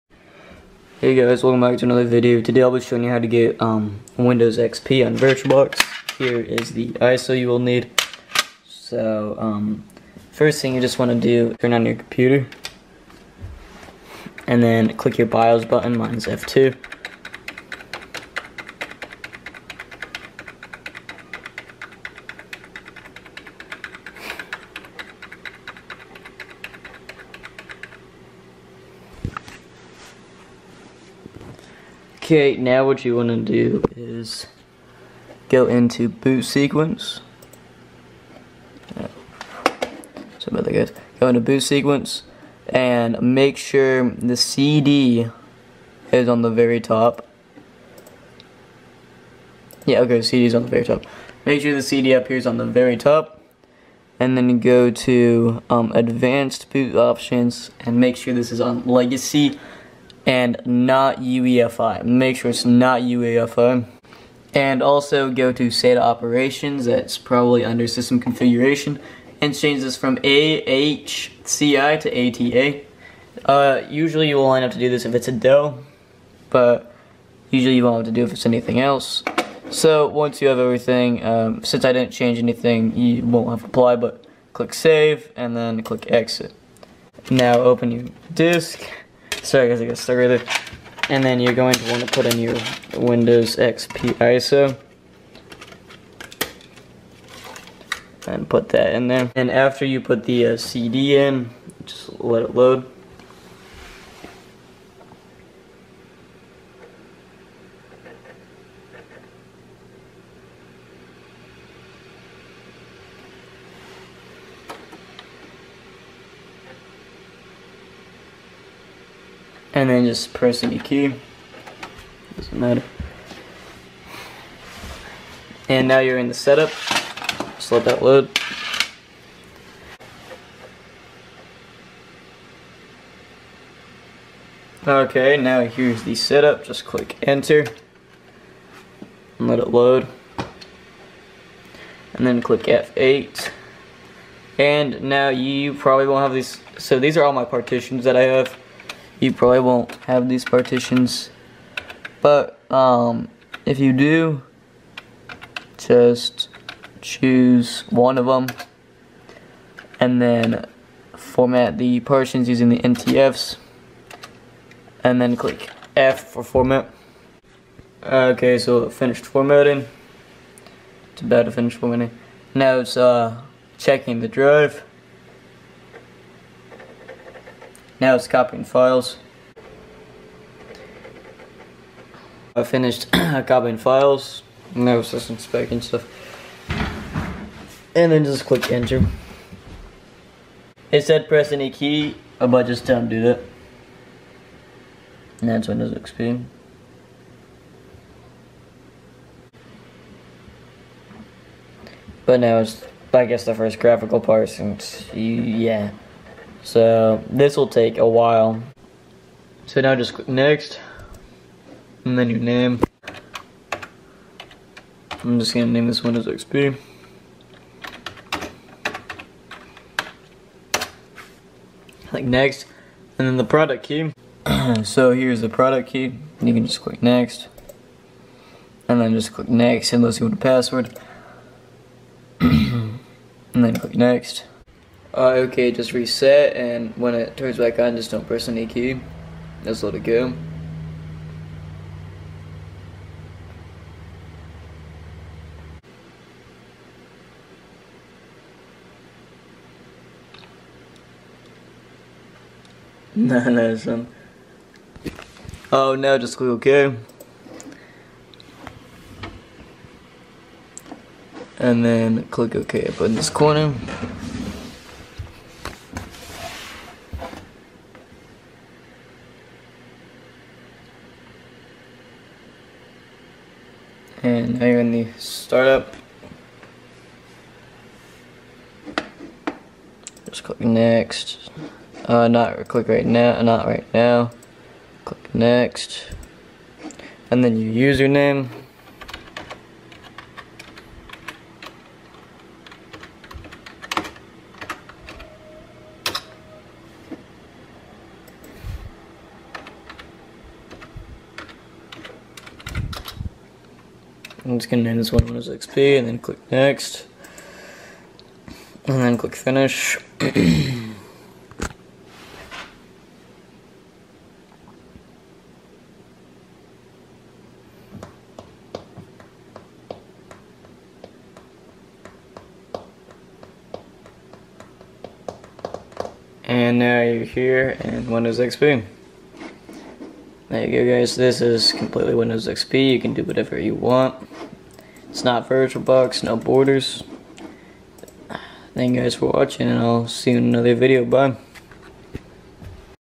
How to get Windows XP on a real Computer
This is a tutorial on how to get Windows XP on a real Computer.
I am using a Dell Optiplex 3020 with a Intel Core i3 8GB of RAM, and a 250GB HDD.
The link to the Windows XP ISO is pinned in the comments.
If you need help with anything els, just comment and I will answer you as soon as possible